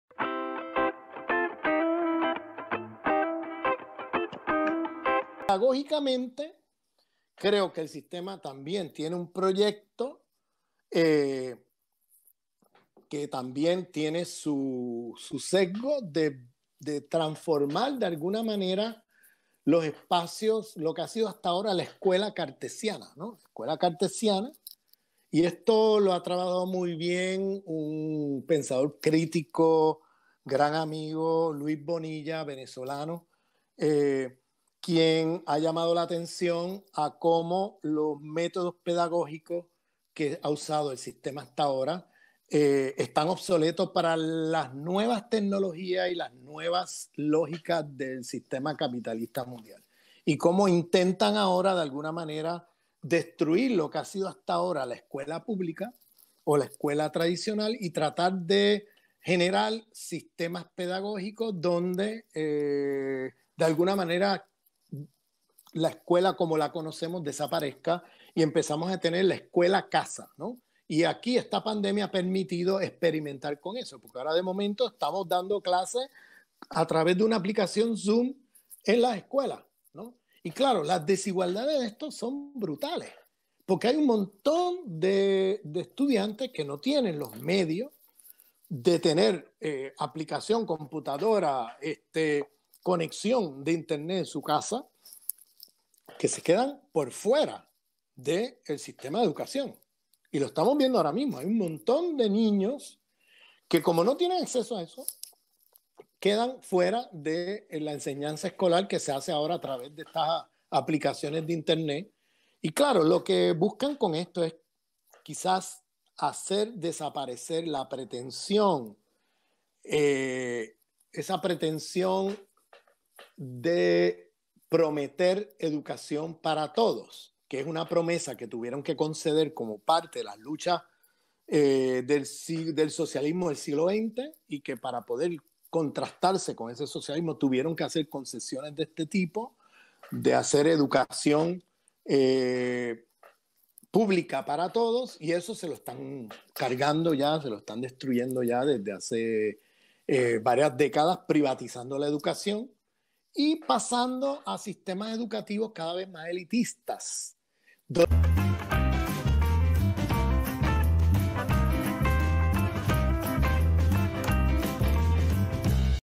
El sistema civilizatorio tiene un proyecto pedagógico - Ramón Grosfoguel
El sistema civilizatorio tiene un proyecto pedagógico.

Este video es parte de la charla con Ramón Grosfoguel (sociólogo, perteneciente al grupo de Modernidad / Colonialidad de la Universidad de Berkeley California) en la serie: Diálogos en medio de la pandemia.